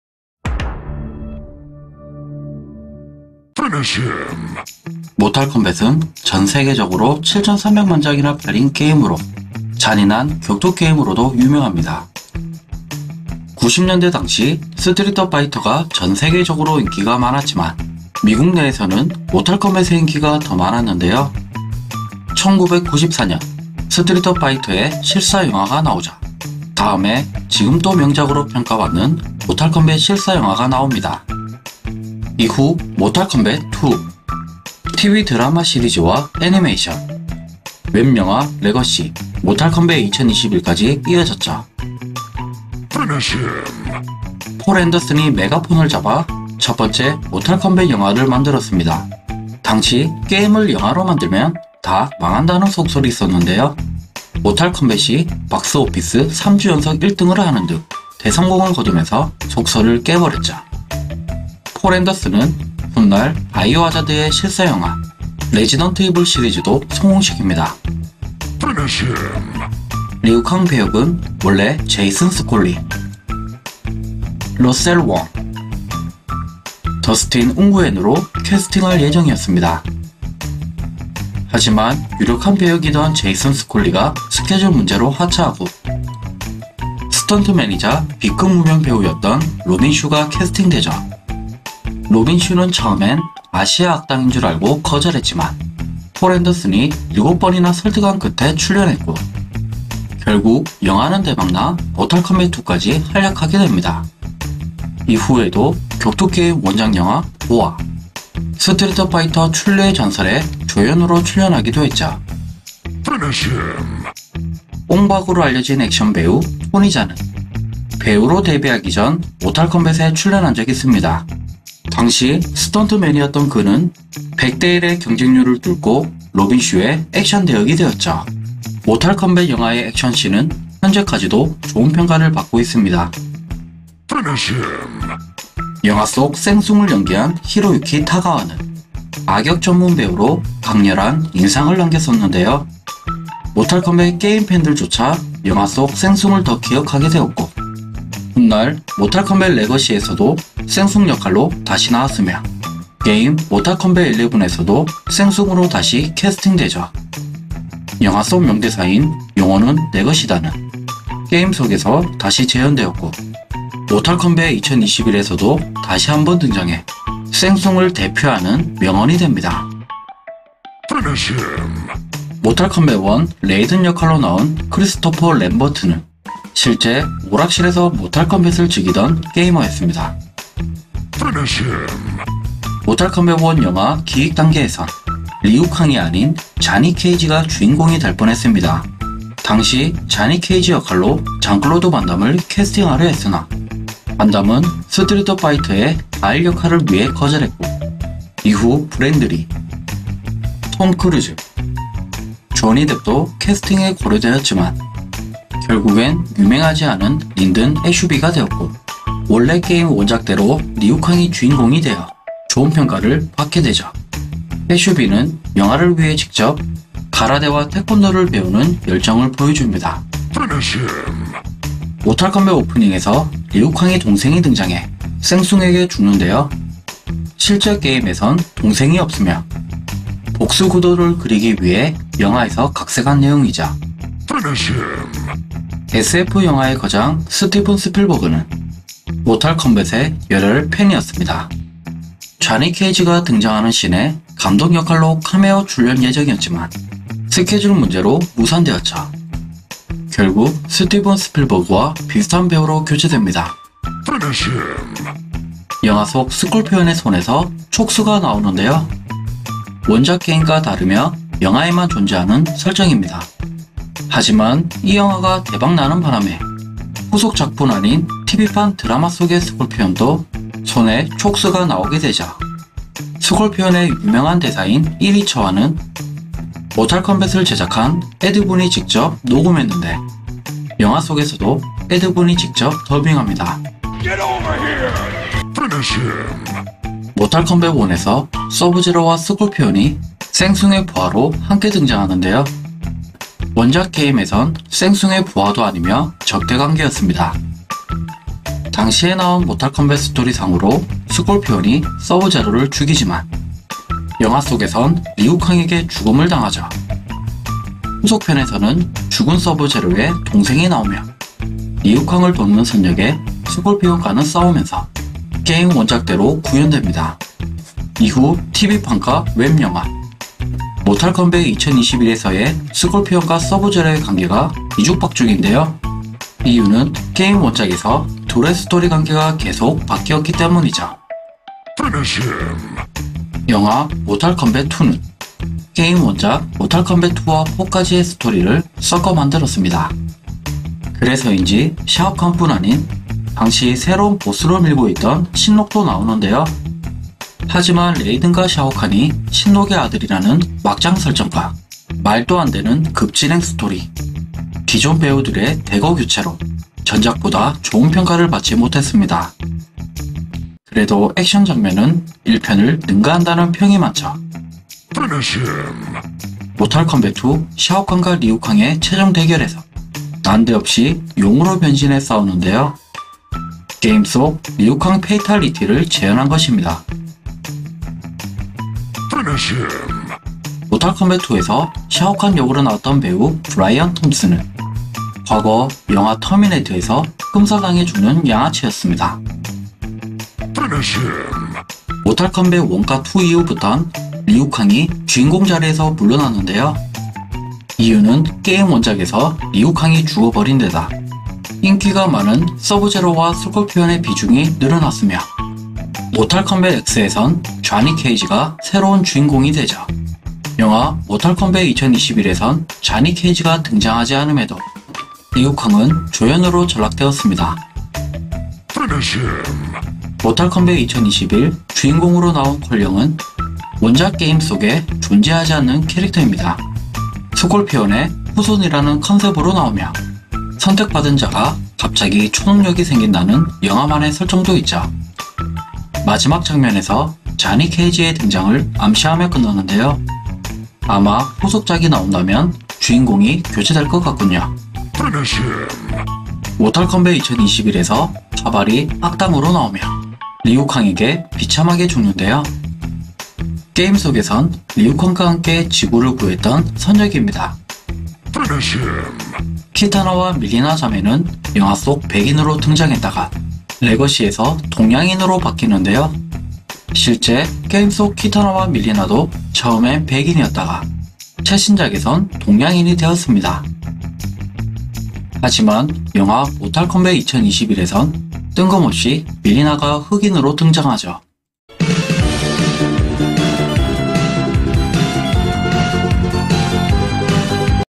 우리가 몰랐던 '모탈컴뱃 영화' TMI 18가지 이야기
모탈컴뱃, 비하인드, TMI, 실사 영화, 리뷰